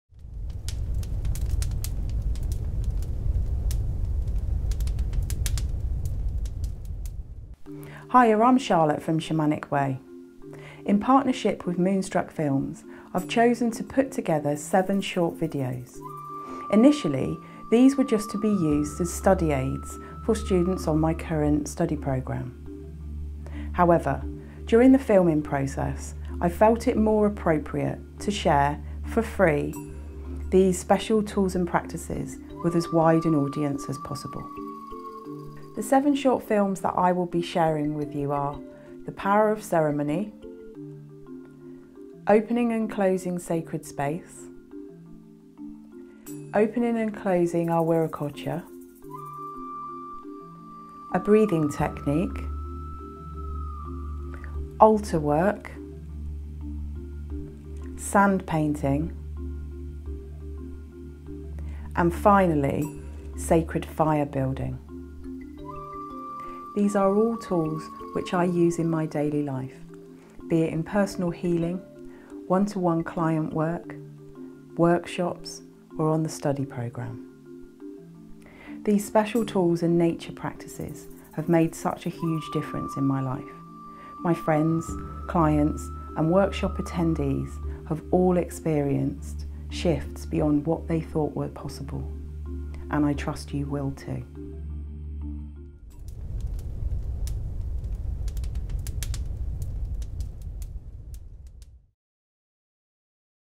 SHAMANIC WAY with Charlotte Gush - Introduction
A brief introduction the Shamanic Tools and Practices short film series...5 further films coming over the next few week, why not subscribe to this channel to ensure you don't miss any.

This is a step by step series that will enable you to
* Open, hold and close a sacred space
*General information about the power of ceremony
*Opening and closing a 'personal' sacred space - the Viracocha
* A breathing technique to help ground you, clear your chakras and connect to the spiritual realms
*Altar building
*Sand painting (nature practice)
* Sacred fire ceremony

Following these teachings, one film at at a time, and bringing them into to your life will enable shifts in perception, daily life, physical health, relationships and so much more. It all depends on which difficulty you wish to work with.

(THIS WORK IS FOR THE INDIVIDUAL, NOT TO INFORM OR AFFECT OTHERS)